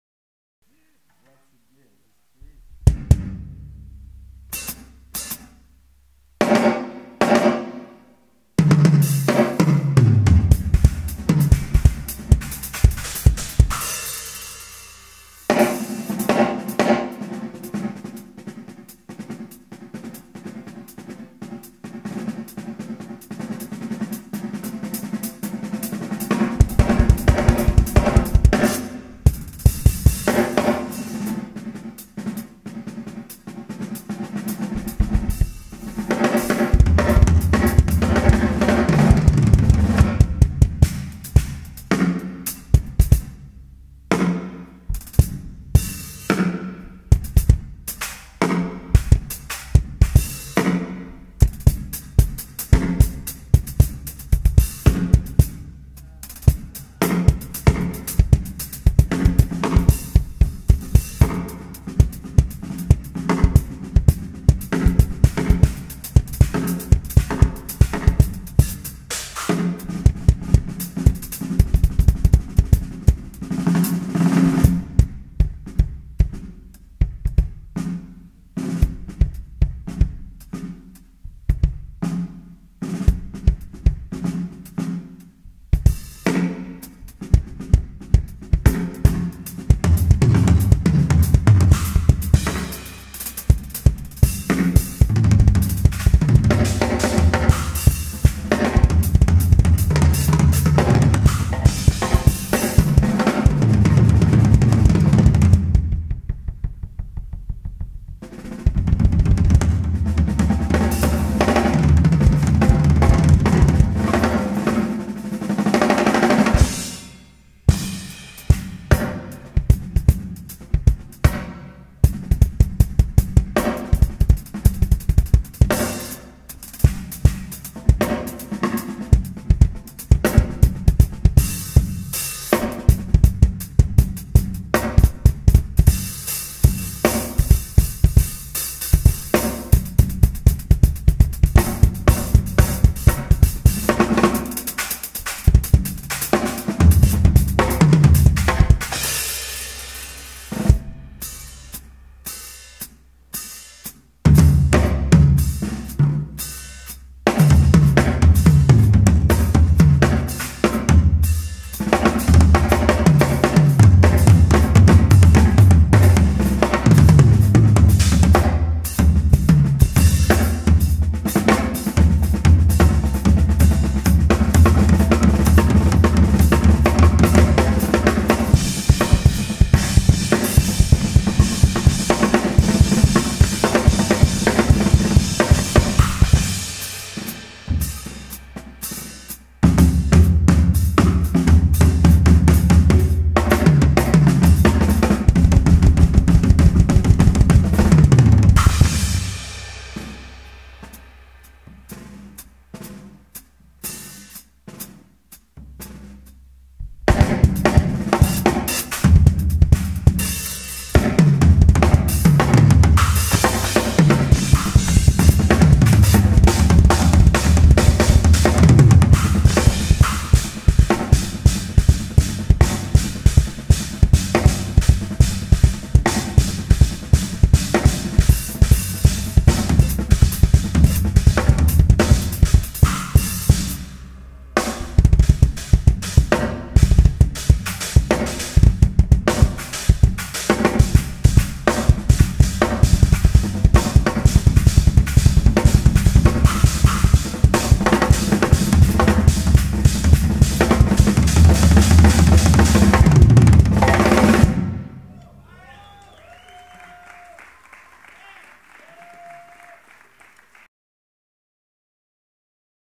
Tony Royster Jr. - Drum session - WikiDrummers 2016
Tony Royster Jr, DW drummer. Drum solo @WikiDrummers 2016 - Part 1 of 6